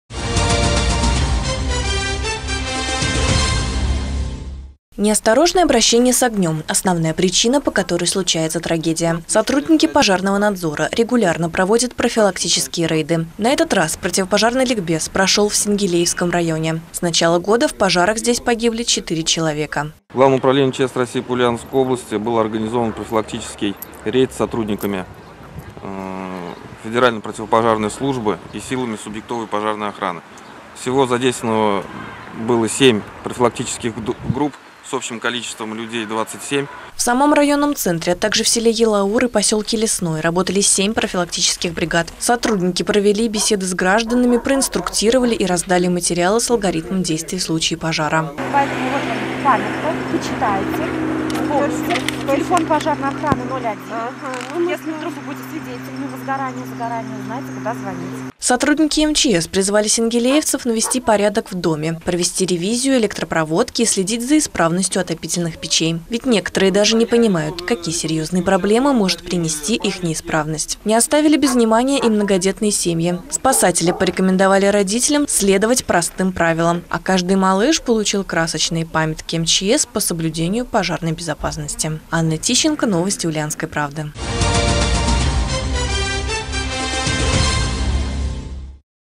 Сотрудники МЧС зашли в гости к сенгилеевцам и проверили проводку
Неосторожное обращение с огнем – основная причина, по которой случается трагедия. Именно поэтому сотрудники пожарного надзора регулярно проводят профилактические рейды.